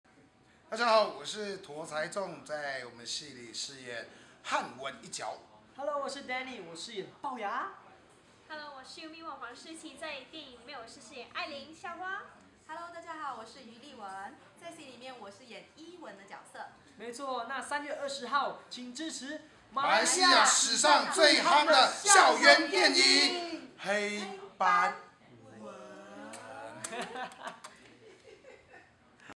黑斑吻 Mokissu - Danny 許佳麟, YumiWong 黃詩棋, Toh Chai Chung 陀財仲 & Lilian 余例紋
黑斑吻 Mokissu Movie Press Conference